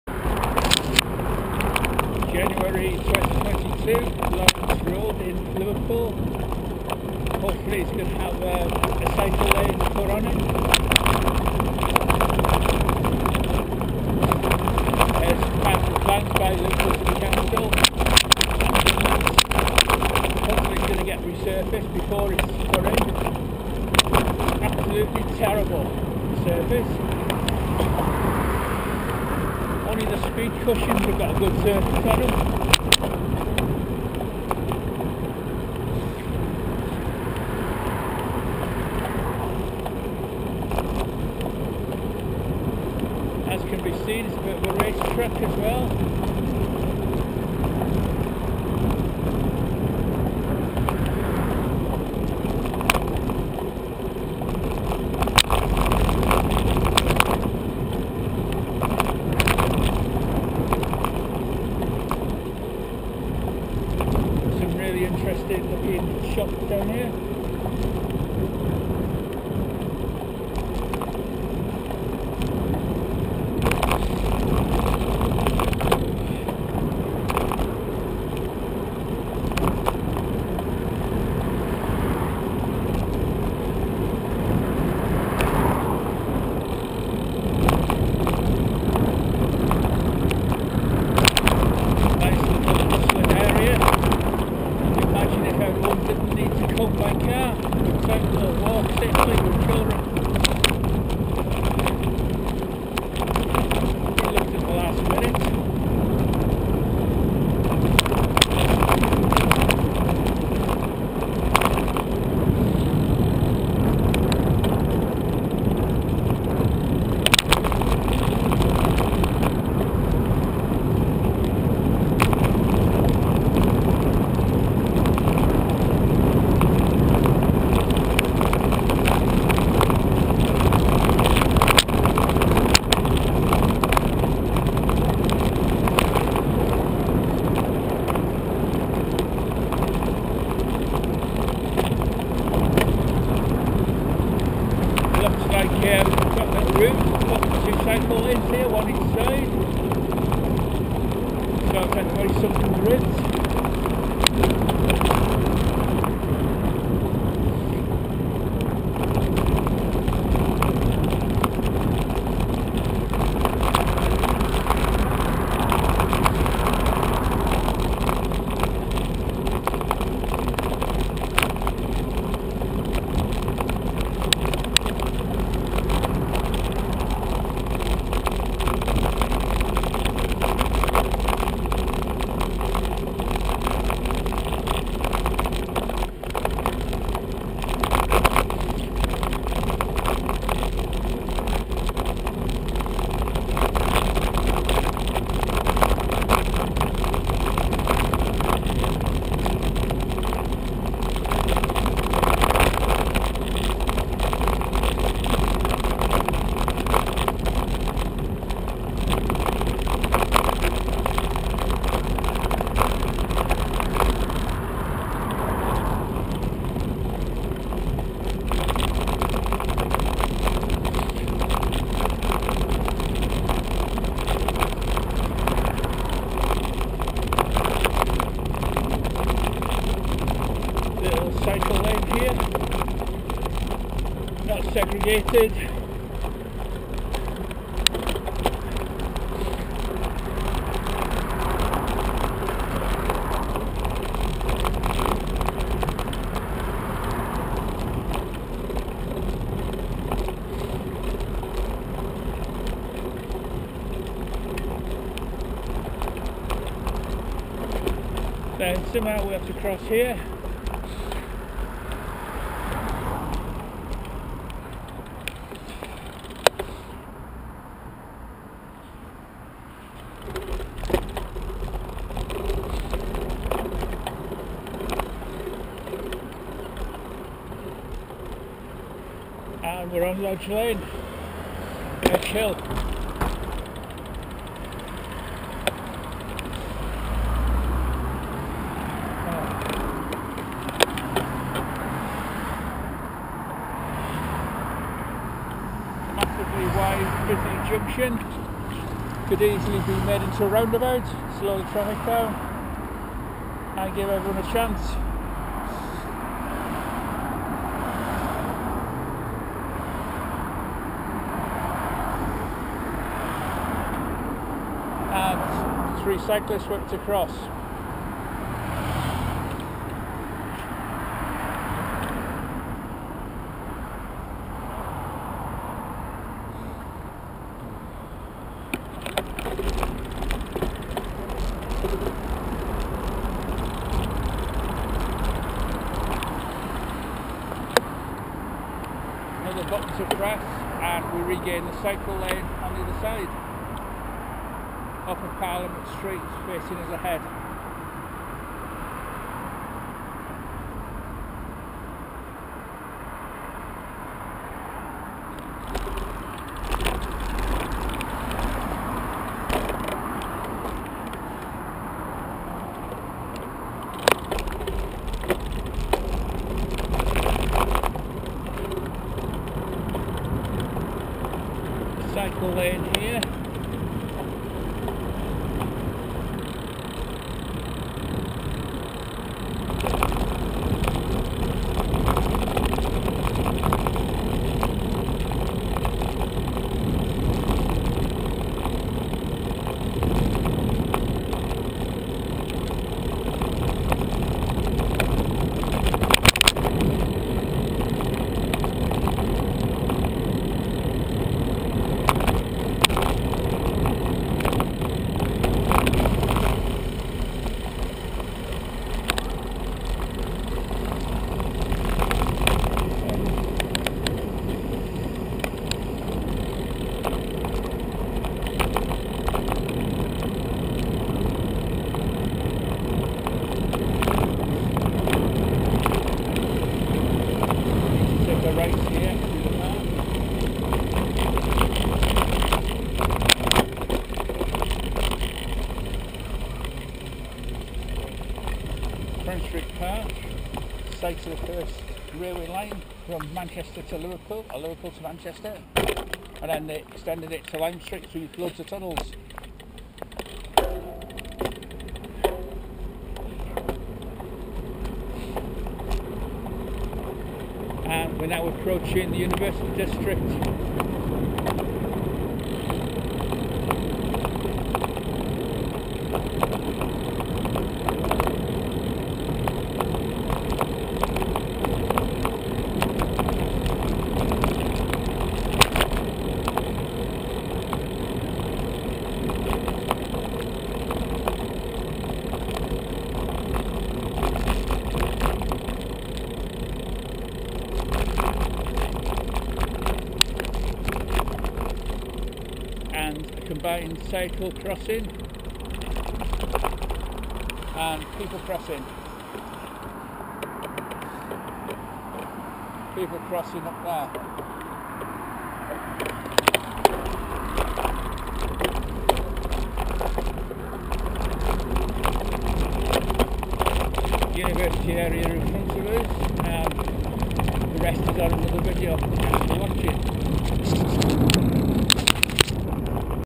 Lawrence road, Liverpool Liverpool Jan 2022
We leave near Wavertree Mystery Park and head up Earle rd to Wavertree an ideal road for single laneing with a bi directional cycle lane down one side, and parking on one side or even both sides where space allows, this road needs proper resurfacing and the end junction needs to be improved  with proper  timings on the traffic lights to link in with the University area of Liverpool.
It would increase footfall for the shops as people would walk or cycle and feel safer and also reduce the speed of traffic, a win win for all.
Its just been announced (Jan 2022) that Liverpool city council are going to put a cycle lane down this road hopefully both sides